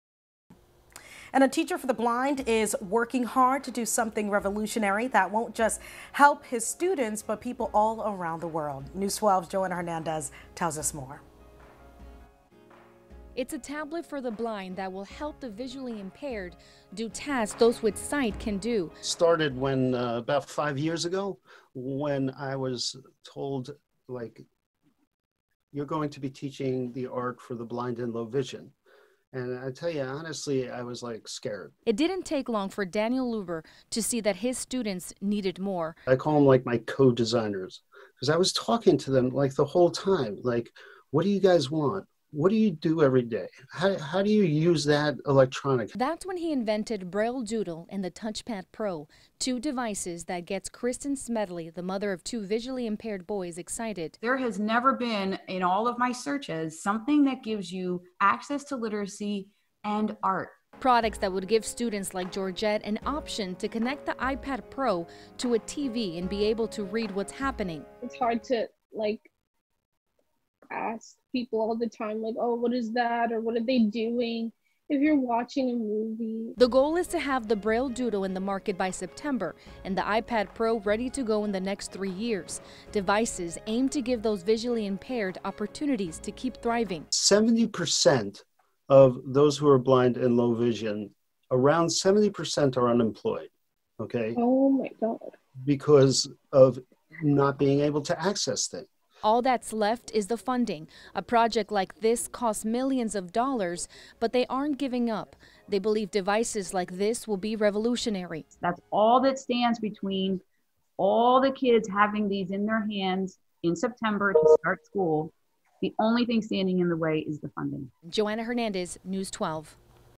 BrailleDoodle inventor: Tablet for the blind is a ‘revolutionary’ teaching tool
A teacher at the New York Institute for Special Education in the Bronx says his invention can be a revolutionary teaching tool for students around the world.